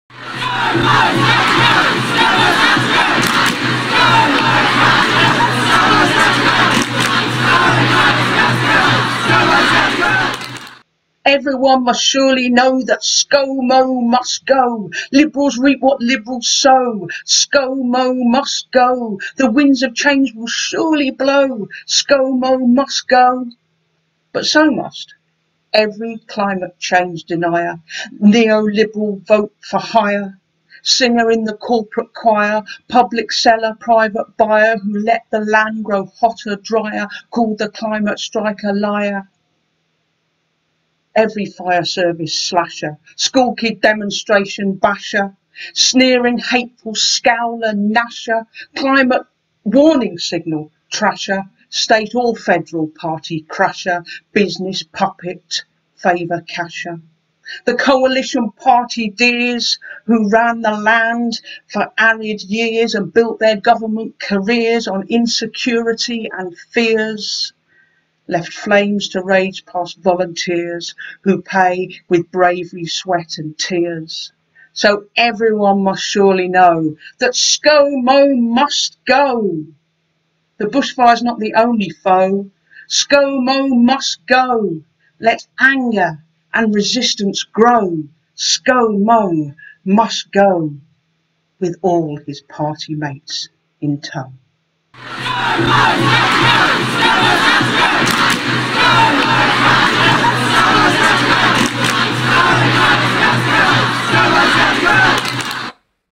Janine Booth: Raging about the Australian bushfires
'ScoMo Must Go!' chants demand the end of Scott Morrison's tenure as Australian Prime Minister. But Janine's poem argues that it is not just ScoMo who must go, but "Every climate change denier, Neoliberal vote for hire ...".